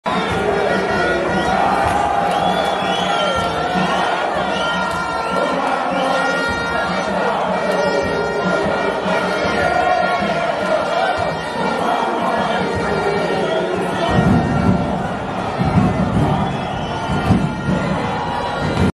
OrlandoPirates fans fighting at FNBStadium.
While other OrlandoPirates fans were celebrating their win against KaizerChiefs at FNBStadium; others were clapping someone.. NedbankCup Semis @orlandopiratesfans2600